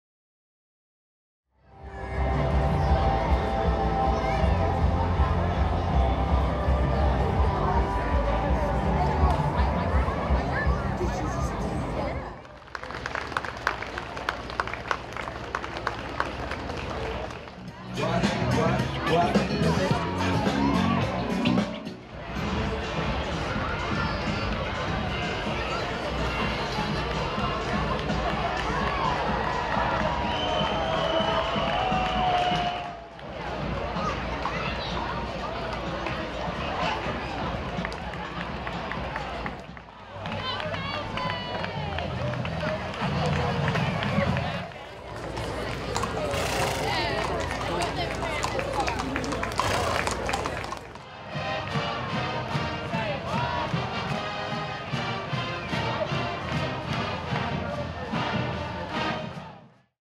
CSU Homecoming Parade 2018
CSU's annual homecoming parade took place earlier this Fall.